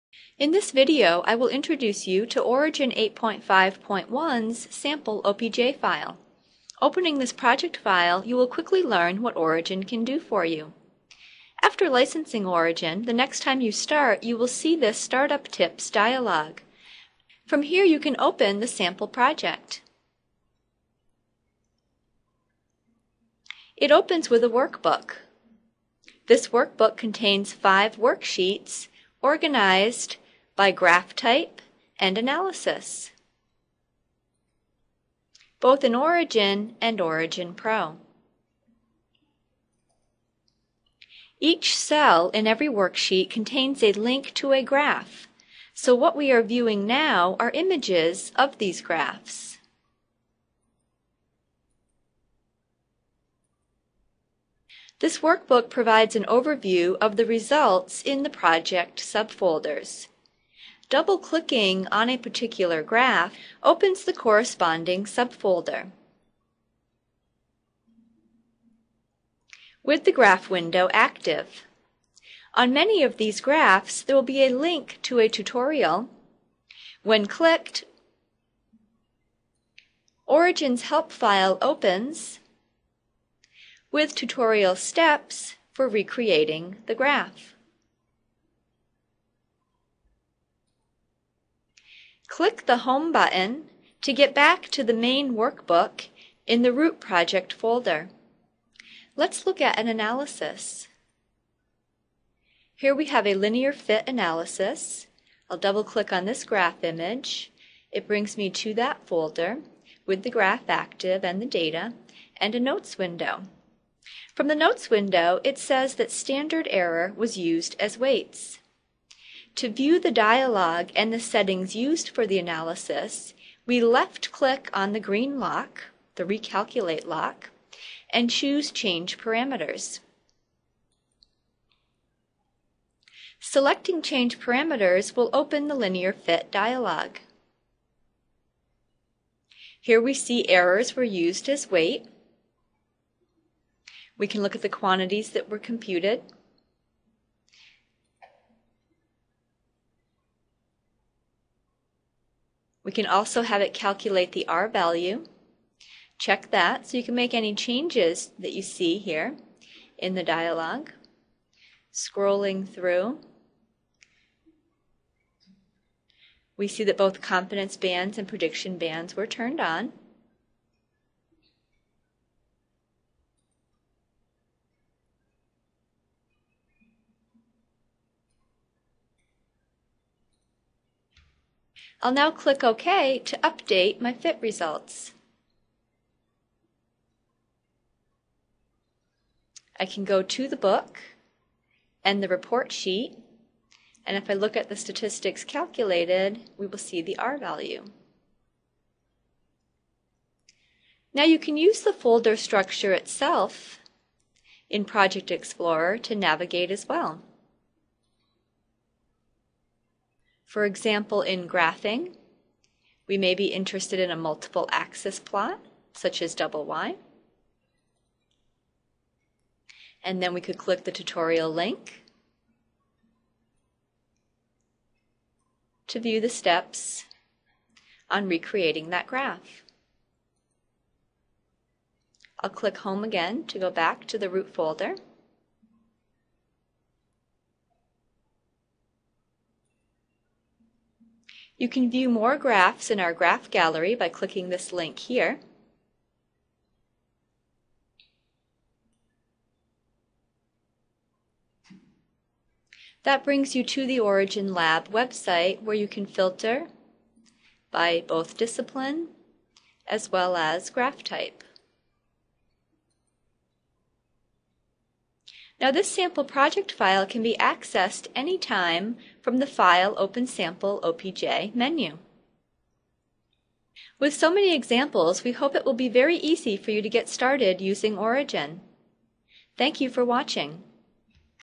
OriginLab® Origin 8.5.1: Sample OPJ
This video shows how to use Origin's new Sample Project file to quickly get started with Origin.